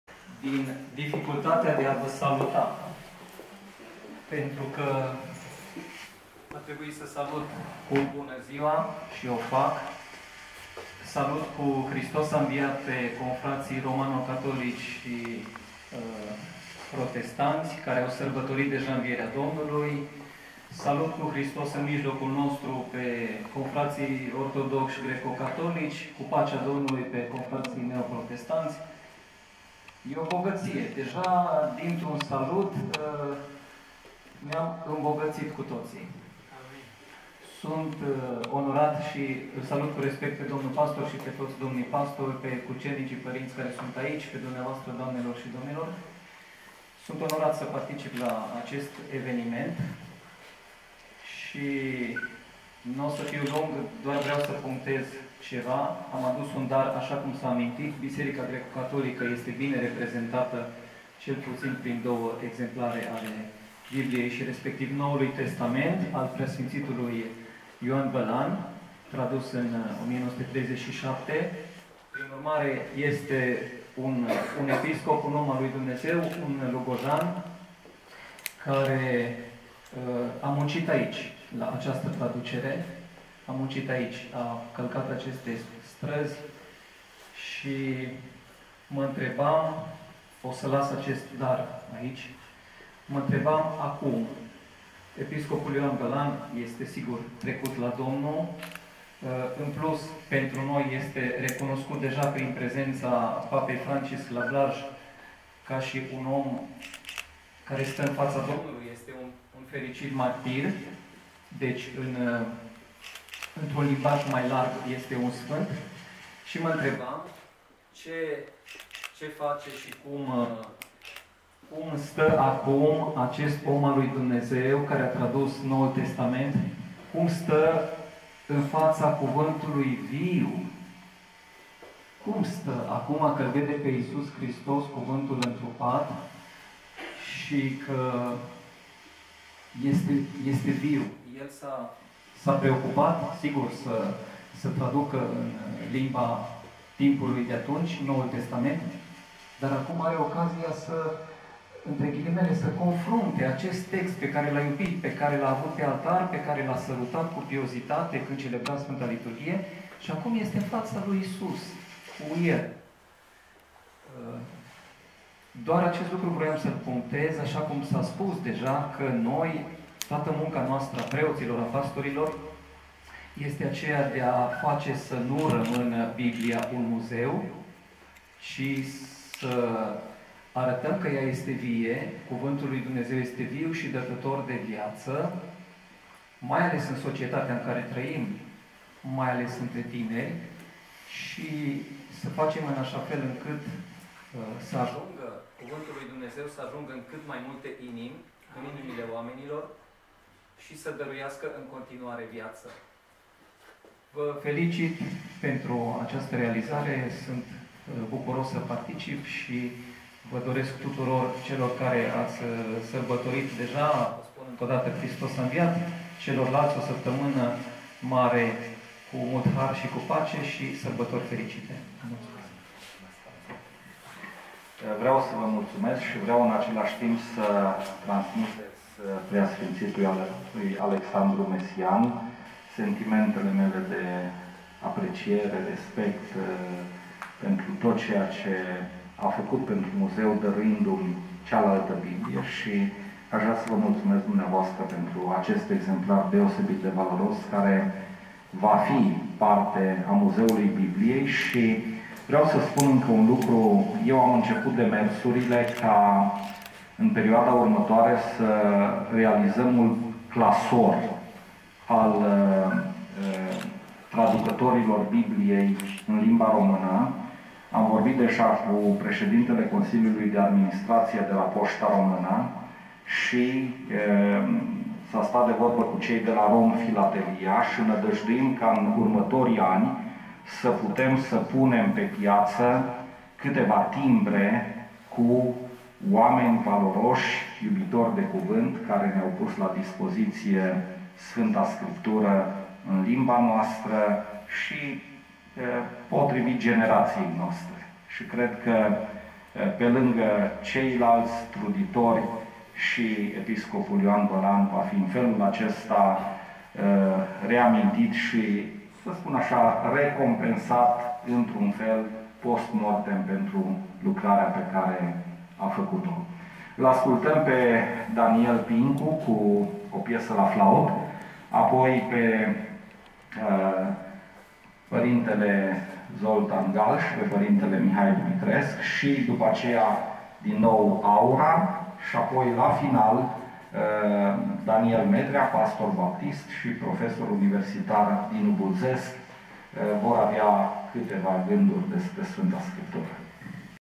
„Muzeul Bibliei”-din colecția Ionel Tuțac -la „Casa Bredicenilor” Lugoj-26 04 2021-v3 Adriana Weimer
„Muzeul Bibliei” -din colecția pastorului Ionel Tuțac -la „Casa Bredicenilor” Lugoj -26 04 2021-video3 Adriana Weimer
„Muzeul Bibliei” - din colecția de BIBLII a pastorului Ionel Tuțac - la „Casa Bredicenilor” Lugoj - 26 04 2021 - video 1-10 Adriana Weimer